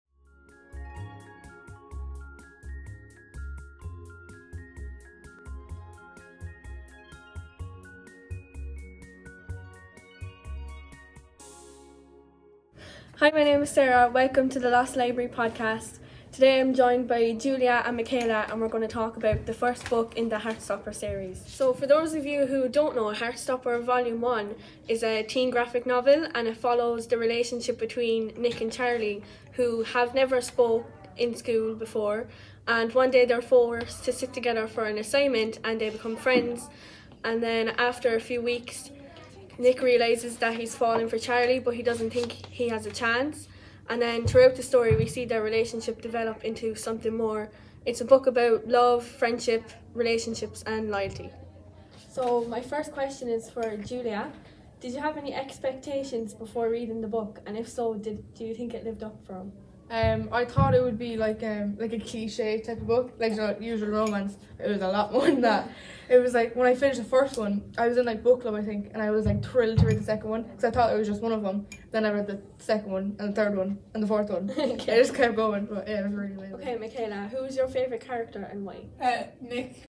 The Lost Library Podcast Podcast
A student created podcast about books by teenagers for teenagers as part of the TY Everyone Can Create Program